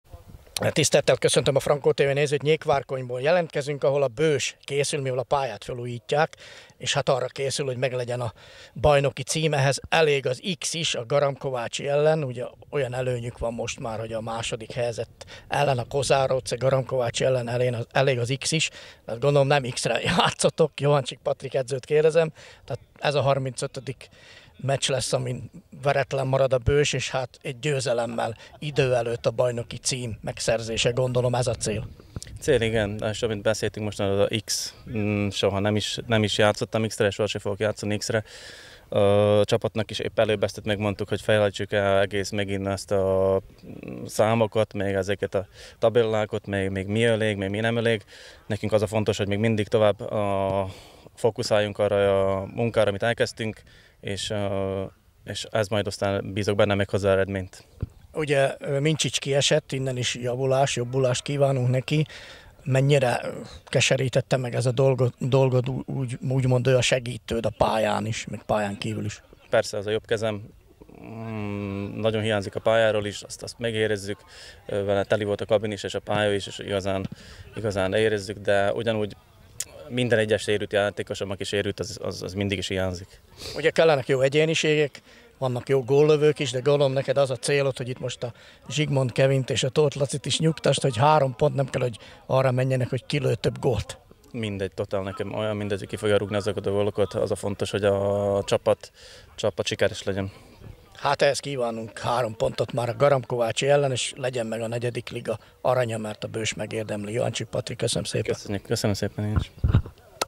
JOHANCSIK PATRIK BŐSI SIKERKOVÁCS:"VERETLENÜL SZERETNÉNK BAJNOKOK LENNI, VASÁRNAP IS NYERNI AKARUNK!
OPERATŐR: ANTALA PISTA. RIPORTER: D. TOK ERNŐ